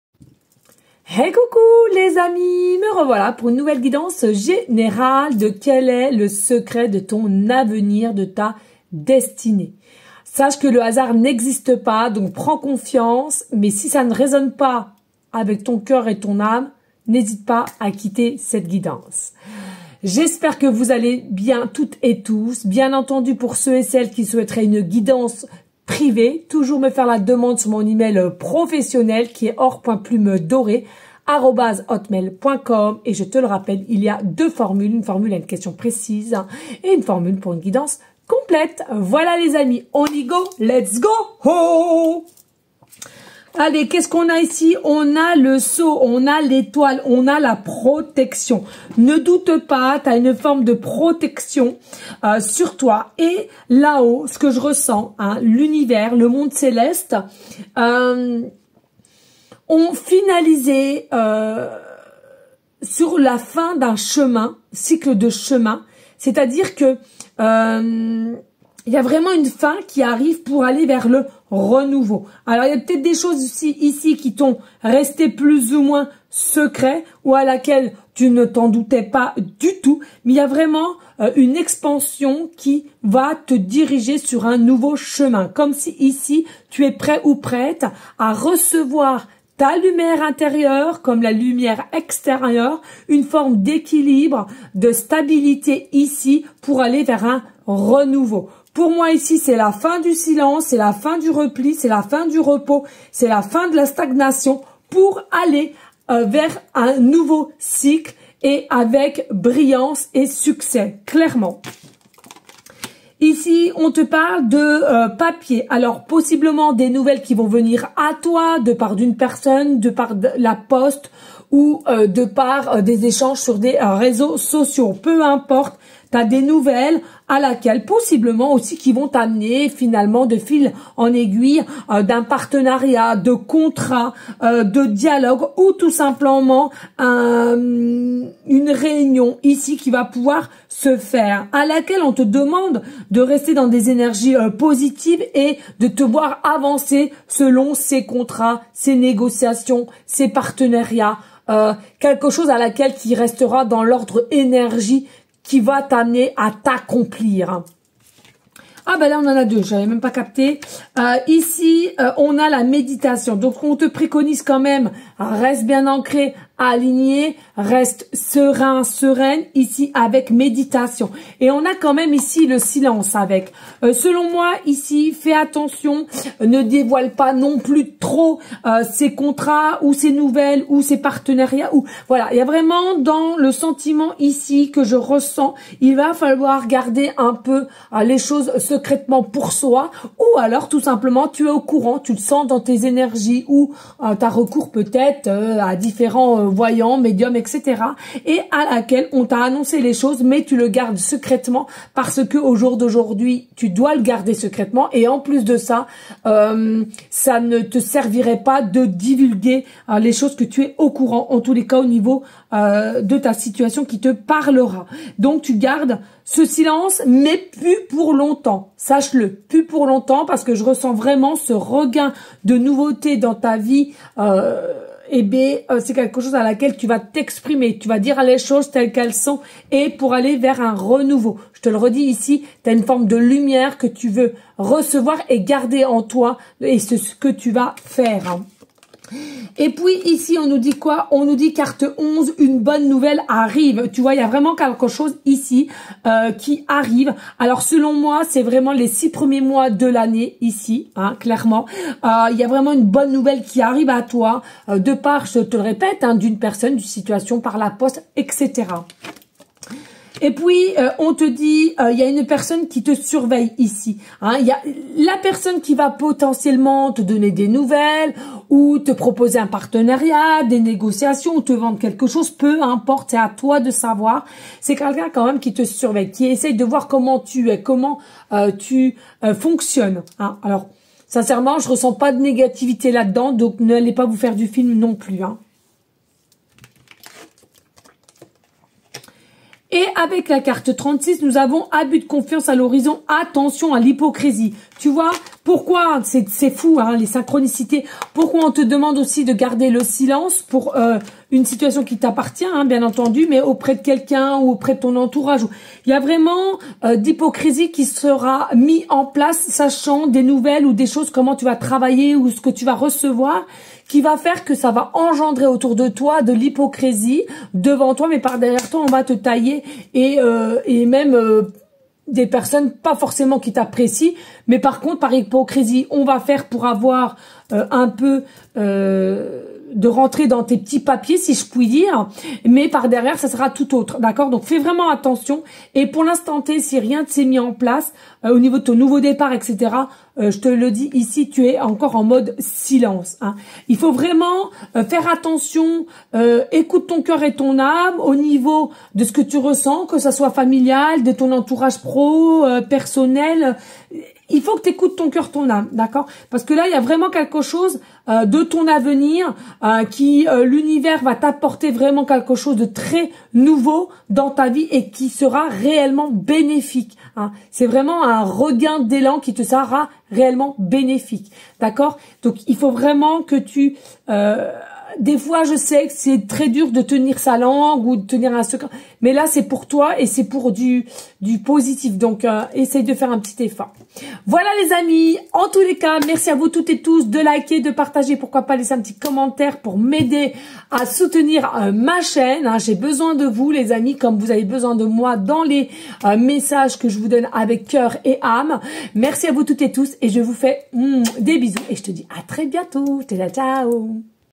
QUEL EST LE SECRET DE TON AVENIR 💕
cela est une guidance GÉNÉRAL et intemporel vous ne prenez simplement ce qu'il vous parles s'il vous plaît 🙏🏻😘❤️

mon adresse pro
société plume Dorée Medium Aurore
19,bis quai Jean Jacques Delorme
41110 saint Aignan sur Cher